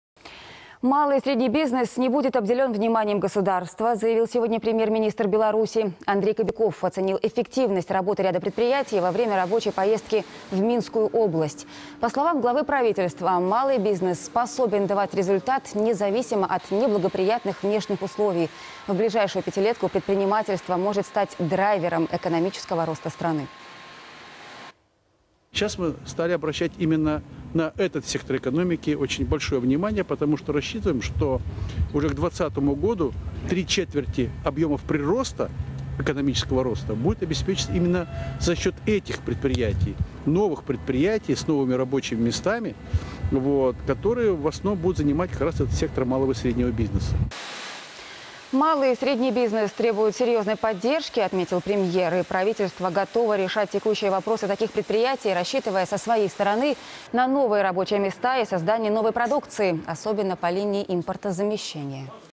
Рабочая поездка Андрея Кобякова в Минскую область
По мнению главы правительства, малый бизнес способен давать результат независимо от неблагоприятных внешних условий.